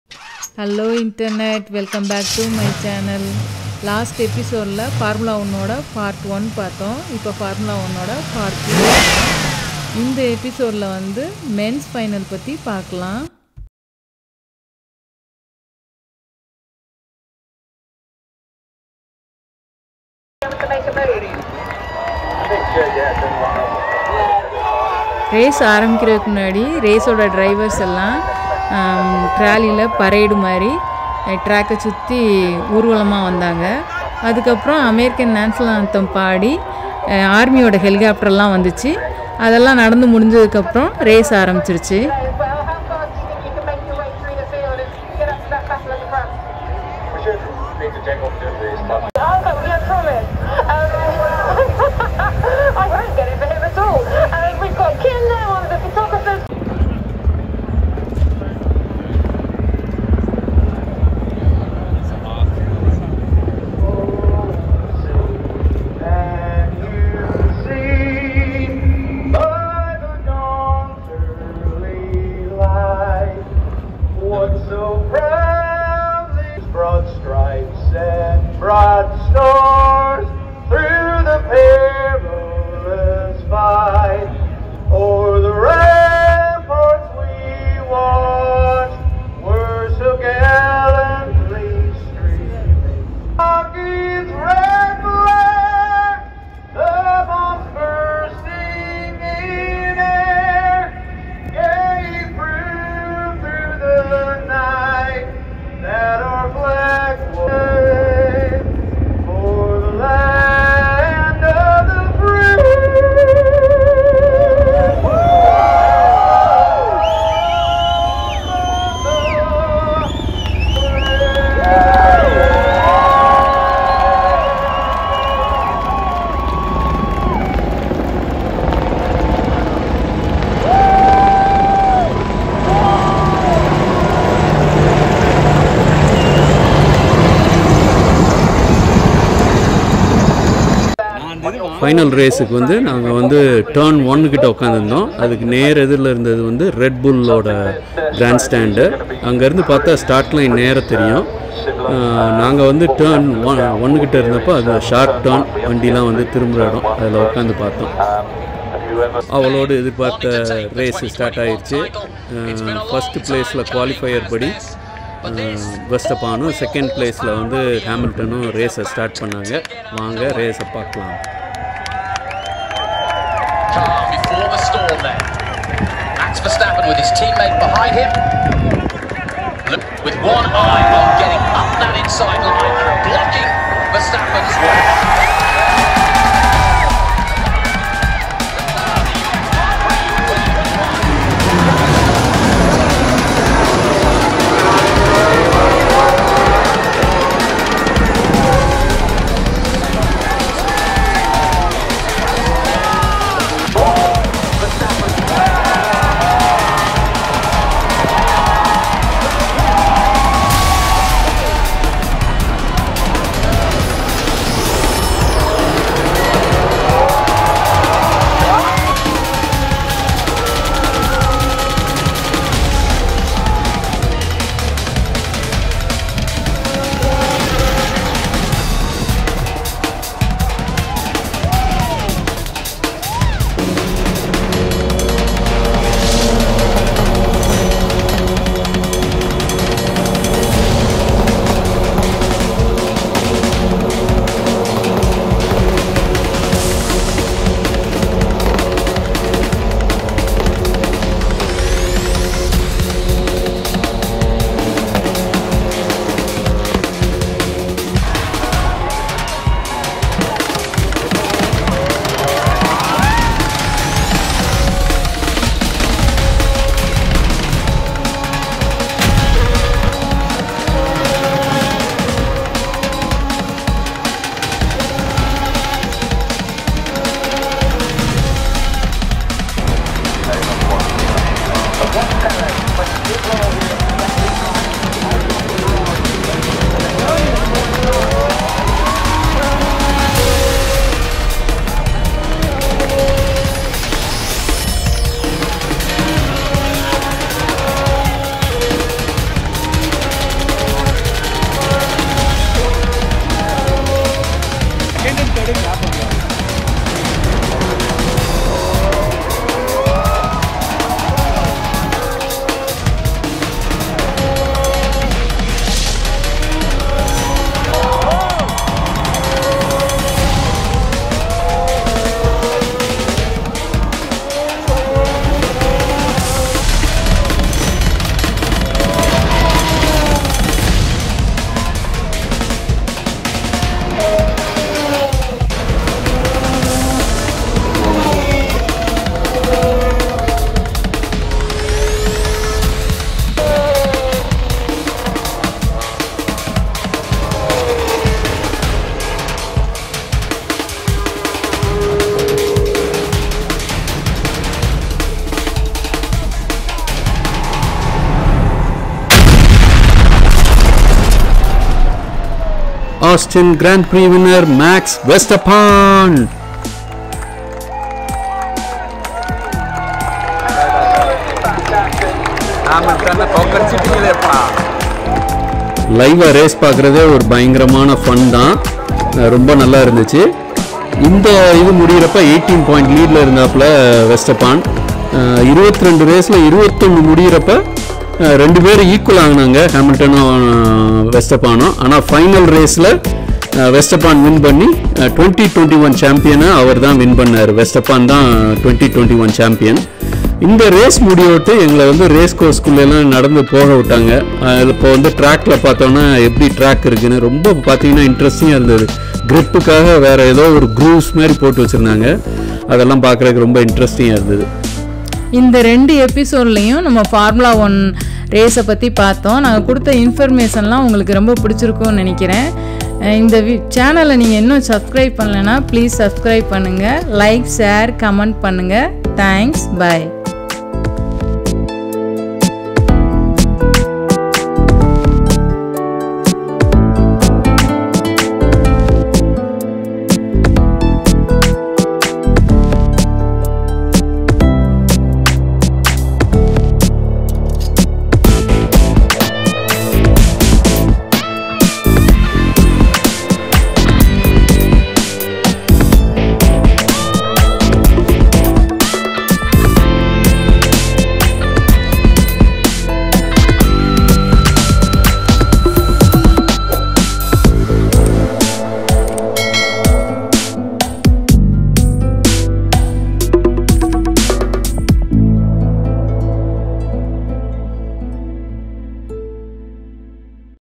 Deepa Travel Vlog - Austin Texas-Ep 4 - FORMULA 1 RACE - Part 2 - Men's Finals-MAX VERSTAPPEN WINS!
We are Tamil Indian Couples living in US.  We are sharing our experience of our travels, information & tips.
In this new series, we are showing you around Austin, Texas.  We watched Formula One Race & visited places there. Formula race was very interesting and was a 3 day event; with day 1 was practice rounds & Womens F2 qualifier; day 2 was Womens F2 Finals & Mens F1 Qualifier; Day 3 was Mens F1 Finals.  Please check youtube videos from Formula One official site; we cannot get clips due to copyright laws.  Our clips are little shacky, but shows what we experienced.  This video is also little noisy; bear with us.